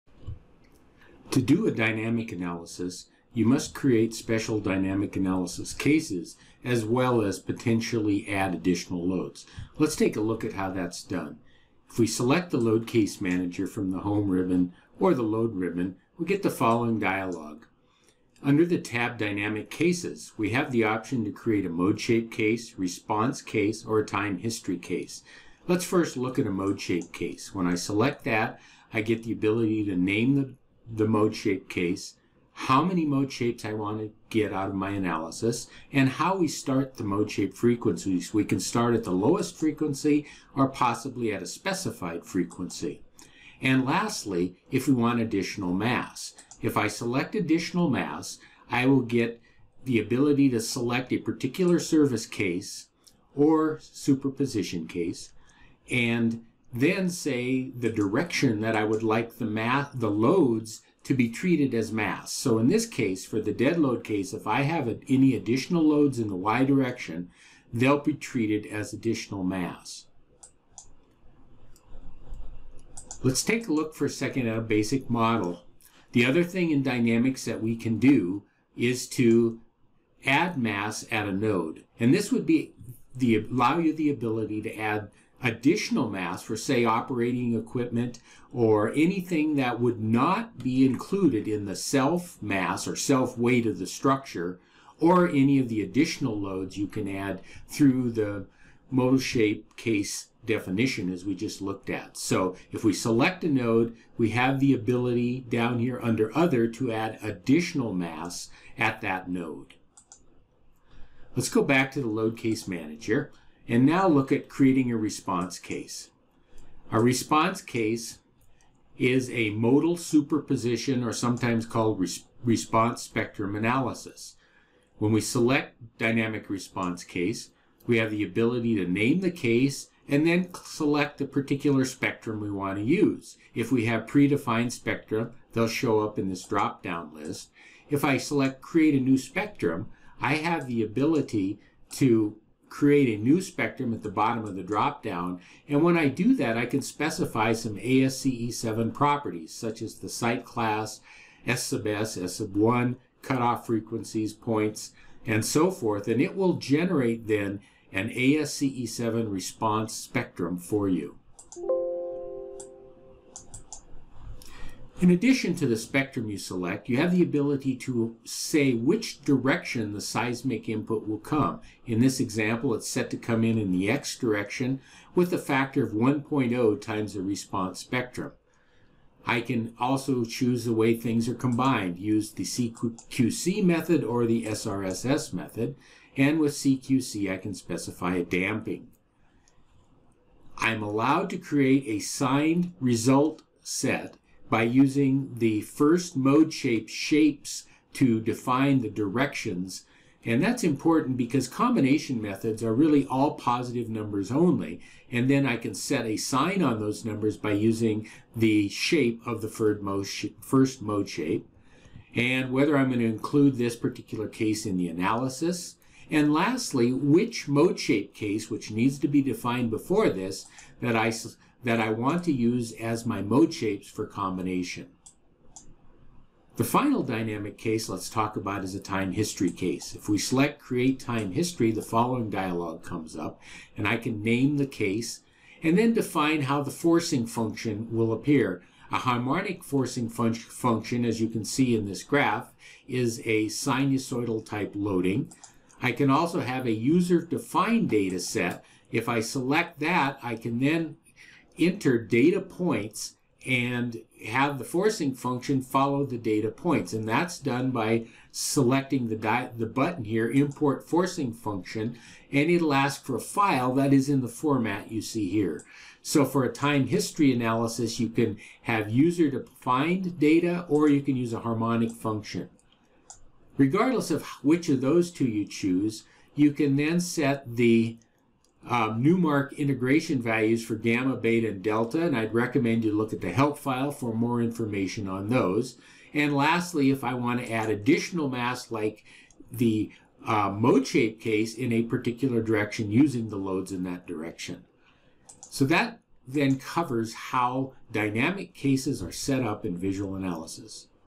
VisualAnalysis: Dynamic Loading
Discusses how dynamic analysis cases are set up.  Mode shapes, modal superposition and time history cases and loads are presented.